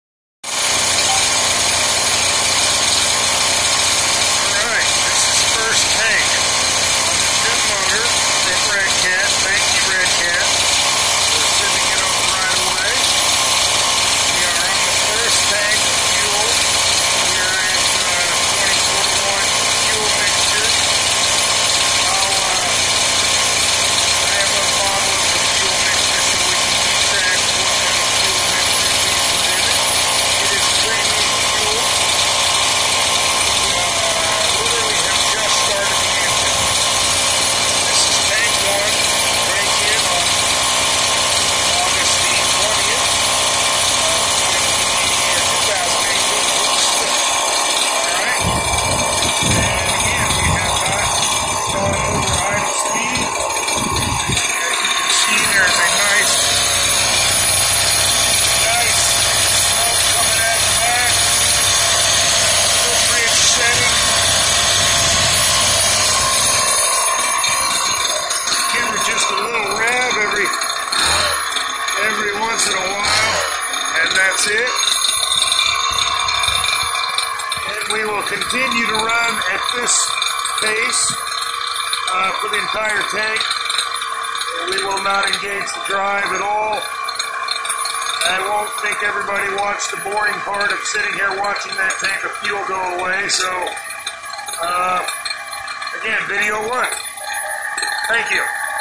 Rampage MT 1/5 scale engine break-in pt1
Breaking in a two cycle engine on an RC car